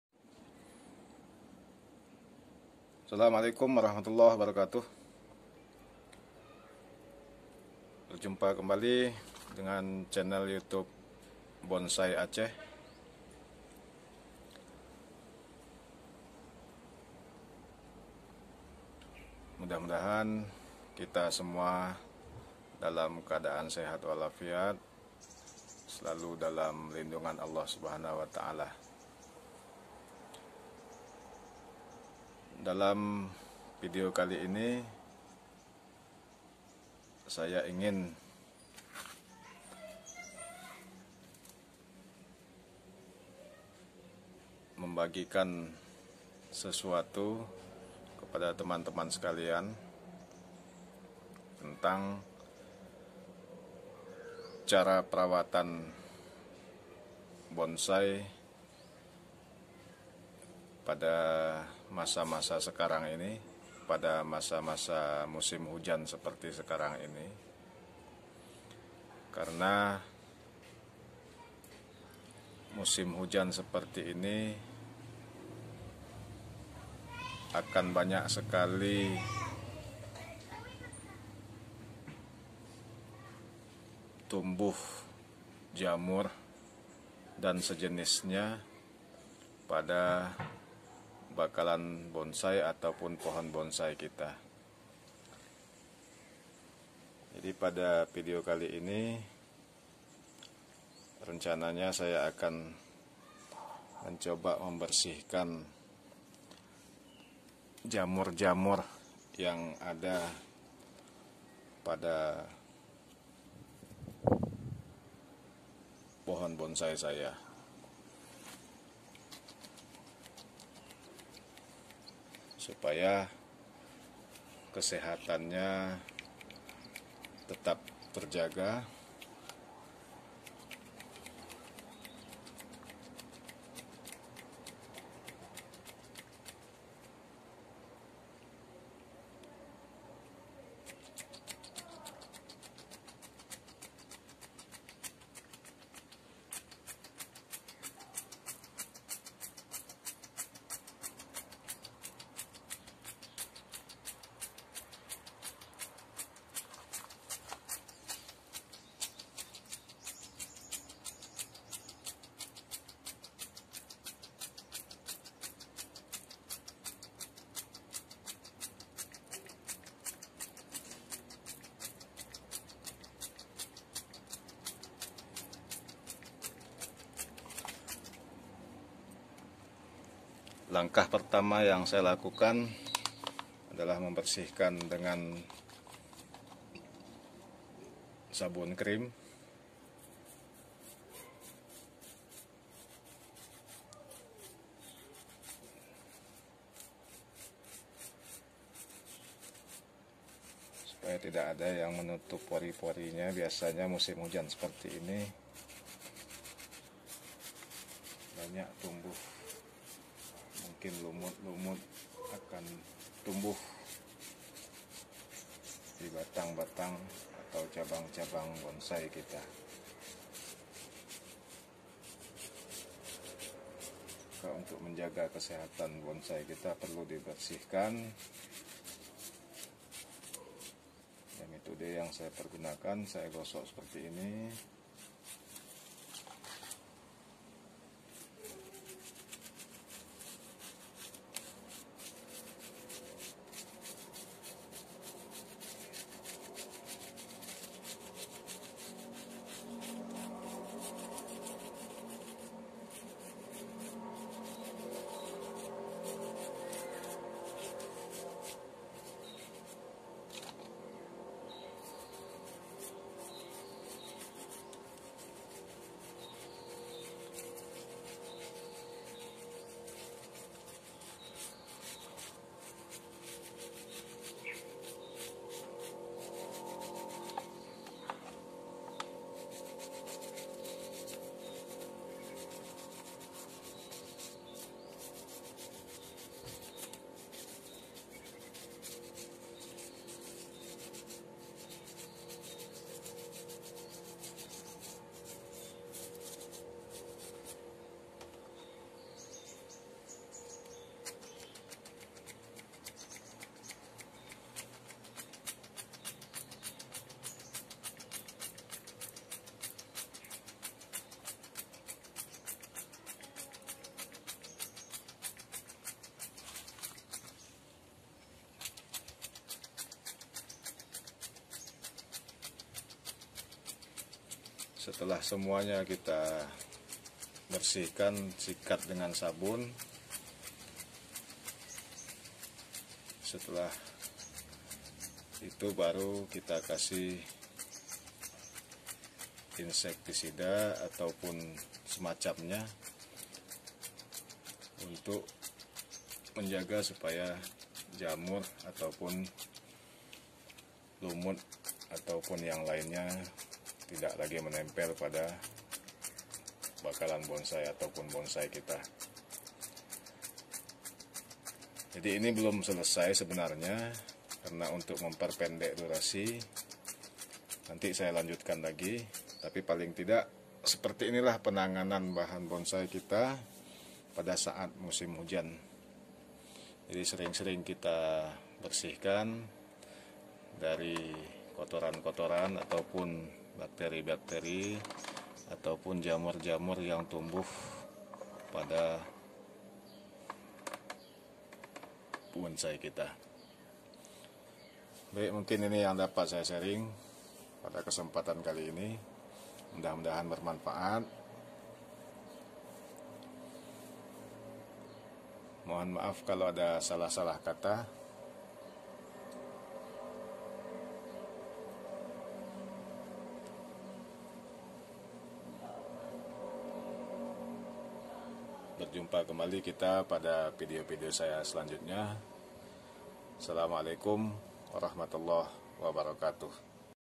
Tips Merawat Bonsai di Musim Hujan
Pada saat musim hujan seperti sekarang ini harus ekstra merawat bonsai. Sekilas tips dari bonsai aceh.